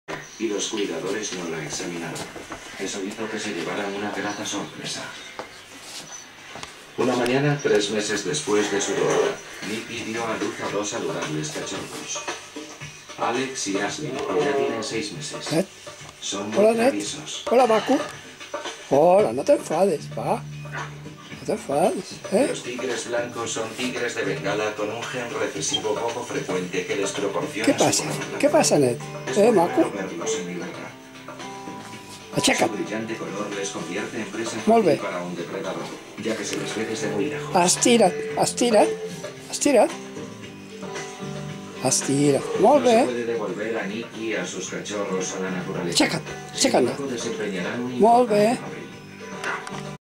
PASTOR DE BRIE UNA RAZA MARAVILLOSA
Mi pero Nett ya ha cumplido 6 meses . Es pues un adolescente,en el video muy breve lo grabo obedeciendo mis ordenes.